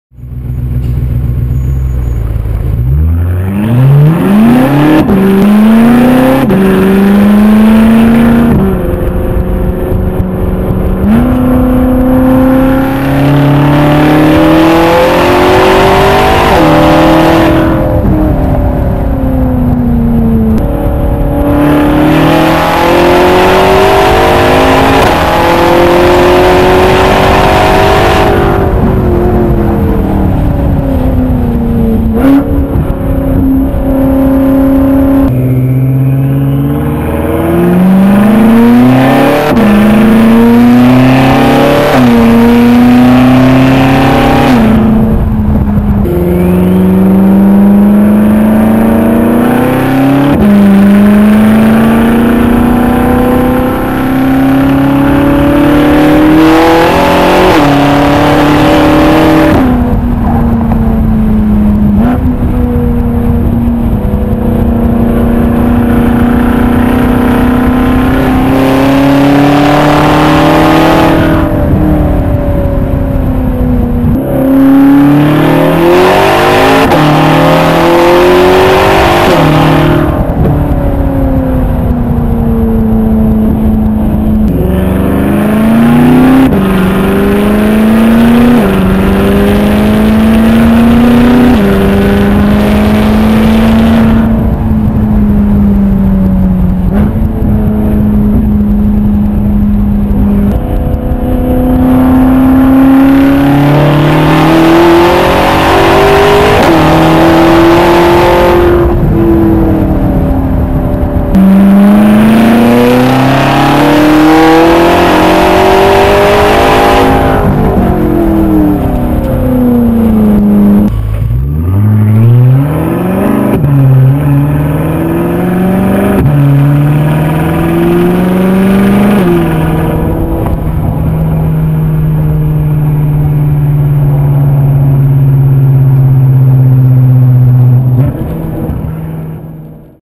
F10 M5 Eisenmann Race Full Exhaust Driving Sound Clips
Some driving sound clips of a 2013 BMW F10 M5 fitted with the Eisenmann Race Exhaust System w/ Center Pipes.